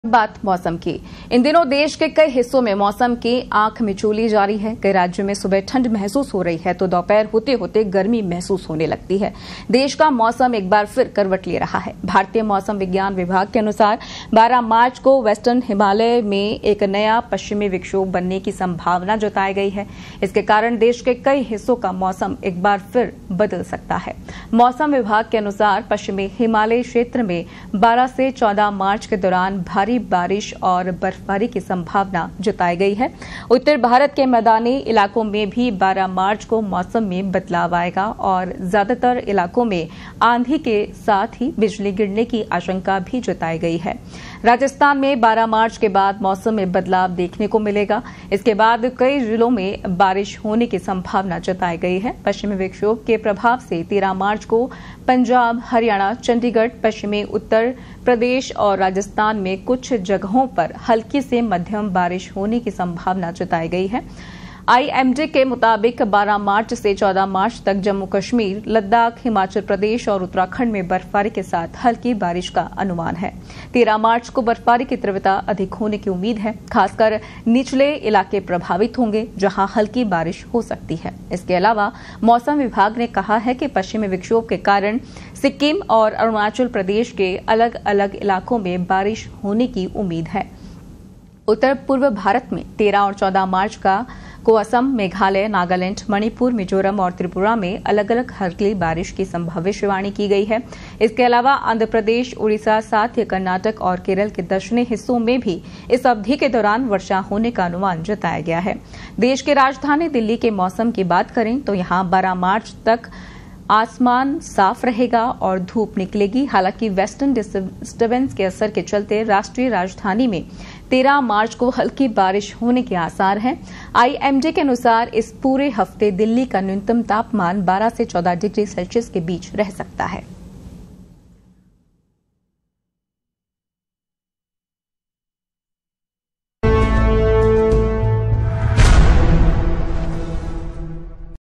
Weather Update Today: बारिश-तूफान का अलर्ट | Delhi-NCR | Weather Latest News | IMD | Breaking News
Weather Update Today: बारिश-तूफान का अलर्ट | Delhi-NCR | Weather Latest News | IMD | Today Mausam Update | Breaking News

इन दिनों देश के कई हिस्सों में मौसम(Weather Update Today) की आंखमिचौली जारी है...कई राज्यों में सुबह ठंड महसूस हो रही है तो दोपहर(IMD Alert) होते-होते गर्मी महसूस होने लगती है...देश का मौसम एक बार फिर करवट ले रहा है...भारतीय मौसम विज्ञान विभाग के अनुसार 12 मार्च को वेस्टर्न हिमालय(Weather Latest News) में एक नया पश्चिमी विक्षोभ बनने की संभावना है, जिसके कारण देश के कई हिस्सों का मौसम एक बार फिर बदल सकता है...मौसम विभाग के अनुसार(Rainfall Alert) पश्चिमी हिमालयी क्षेत्र में 12 से 14 मार्च के दौरान भारी बारिश और बर्फबारी की संभावना है...उत्तर भारत(Uttarakhand Weather Update) के मैदानी इलाकों में भी 12 मार्च को मौसम में बदलाव आएगा और ज्यादातर इलाकों में आंधी के साथ ही बिजली गिरने की आशंका है...राजस्थान(Rajasthan Weather Update) में 12 मार्च के बाद मौसम में बदलाव देखने को मिलेगा, इसके बाद कई जिलों में बारिश(Bihar Weather Update) होने की संभावना है...पश्चिमी विक्षोभ के प्रभाव से 13 मार्च को पंजाब, हरियाणा, चंडीगढ़, पश्चिमी उत्तर प्रदेश(UP Weather Update) और राजस्थान में कुछ जगहों पर हल्की से मध्यम बारिश हो सकती है...देश की राजधानी दिल्ली(Delhi Weather Update) के मौसम की बात करें तो यहां 12 मार्च तक आसमान साफ रहेगा और धूप निकलेगी...